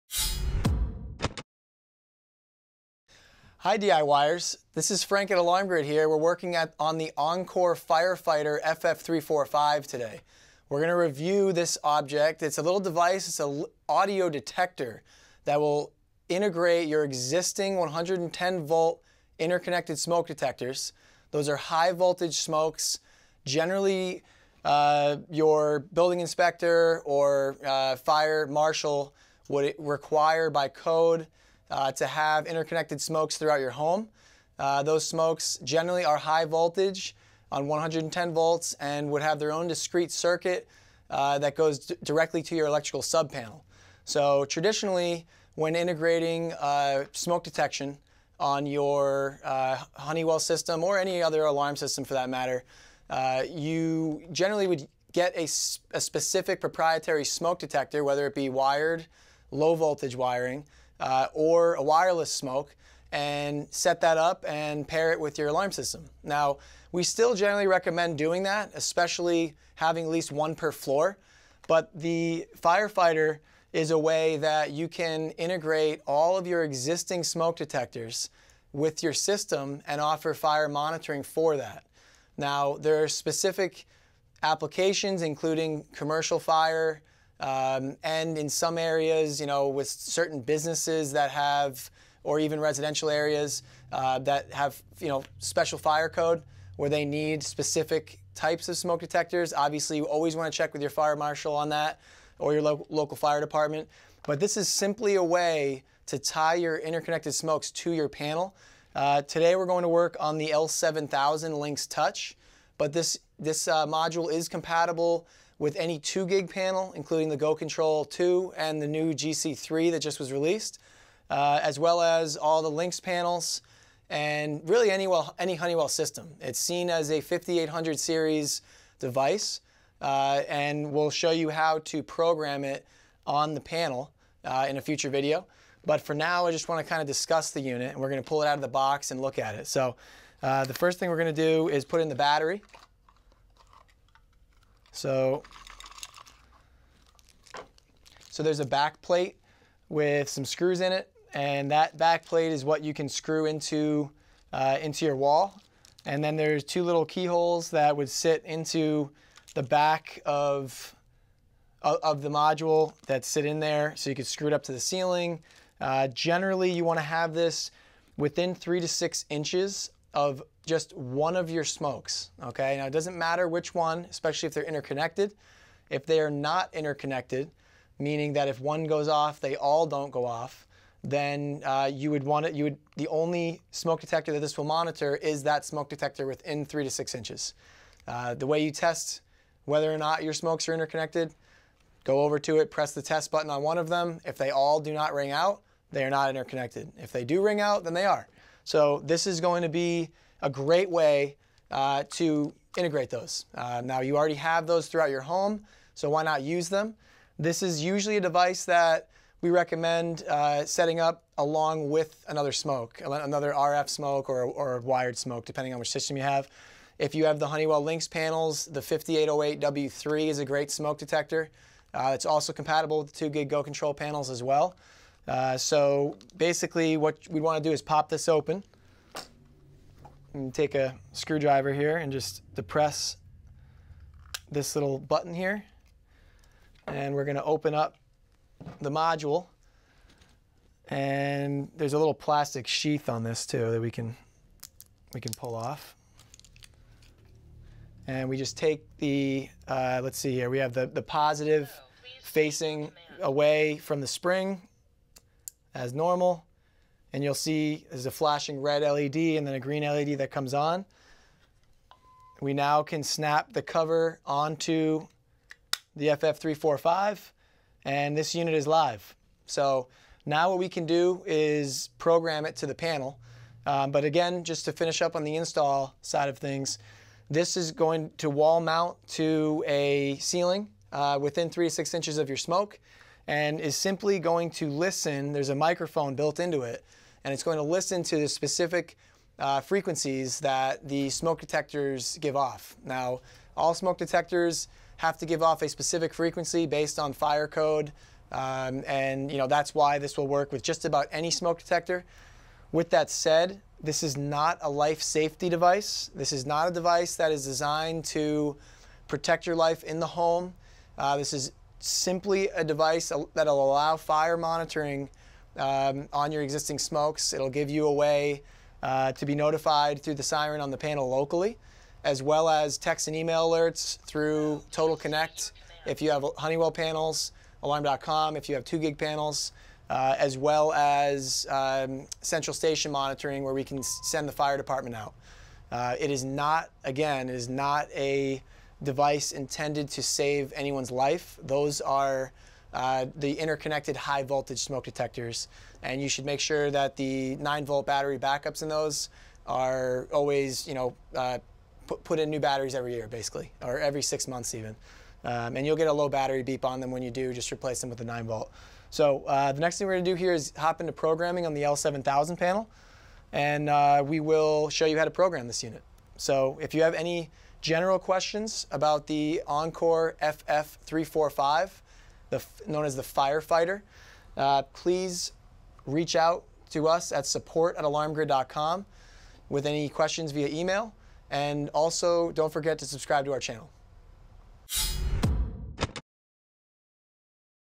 FireFighter FF345: Review
Our video review of the Encore FireFighter FF345 add-on for hard-wired smoke detector integration with your existing alarm system.

The FireFighter FF345 is an audio detector that integrates high voltage, hard-wired smoke detectors. As the video discusses, having one integrated smoke detector per floor is highly recommended. With the Encore FF345 you can integrate all the other previously installed smoke detectors.

Out-of-the-box setup is relatively easy. As you follow along with the tutorial, you need to put in the battery. The FF345 comes with a back plate and mounting screws for easy mounting. As we tell you in the video, the recommended mounting is to place the FF345 on the ceiling within 3 to 6 inches from your smoke detectors.

The limitations of the Encore FireFighter FF345 is that if your smoke detectors are not integrated: meaning they do not all alarm if one rings, then the FF345 will only relay the signal for the smoke detector it is positioned near.

If you have interconnected smoke detectors, you can place the Encore unit near any one you choose. However, if they are independent smoke detectors, you should get an FF345 for each level of your home, if not for each smoke detector. The walk through will tell you more about this and the relative fire codes involved.

The video will show you how to test if your smoke detectors are integrated and hardwired or if they are single operational units. It is a simple method that you can do at any time to find out if you are unsure. Every smoke detector has a test button. You can press this button to check for interconnectivity.

To make the unit live you just have to remove the back panel and install the battery. An LED will flash from red to green to let you know that the unit is operational. In the video instruction, you will learn that the Encore FF345 just needs to be programmed into the alarm system control panel.

The FireFighter FF345 will work with virtually any smoke detector. The video guide explains why. The built-in microphone is designed to listen for the smoke detector alarm frequency specifically. As you will see, once this frequency is picked up by the FF345 microphone, the alarm is sent to the control panel which will pass on the alert to you.

While the video makes it very clear the unit is not a life-saving device, it will cause the alarm system to alert you of an emergency. Through Total Connect you can receive text and email alerts, through the siren on the panel locally, as well as central station monitoring.

You can view our other videos about the FireFighter unit that will walk you through programming the FF345 to your existing control panel. Once that is done all that is left is for you to mount the unit on the ceiling, remembering to maintain the 3 to 6 inches from your smoke detector.